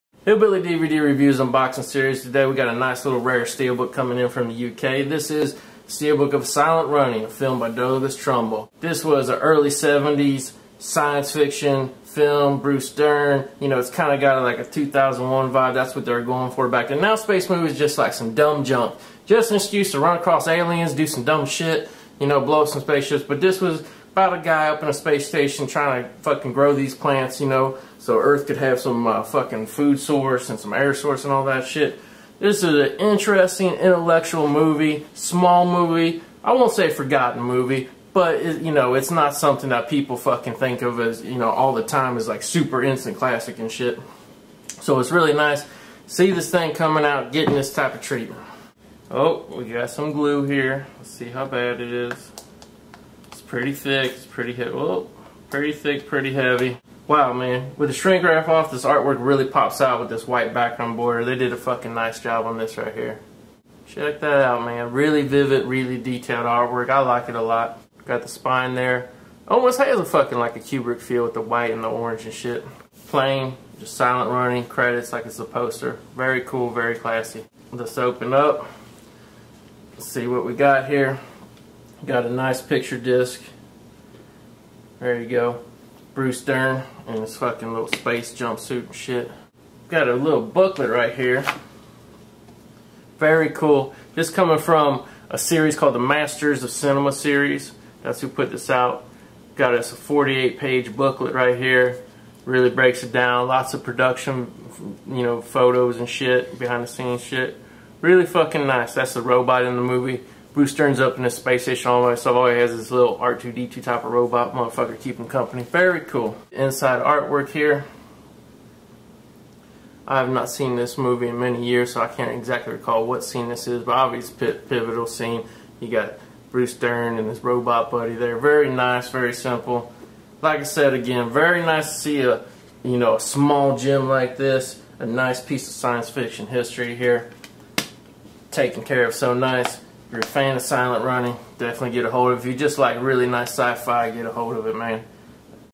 SILENT RUNNING UK blu-ray steelbook unboxing
Blast off with The Goat as he unboxes the UK blu-ray steelbook of Silent Running.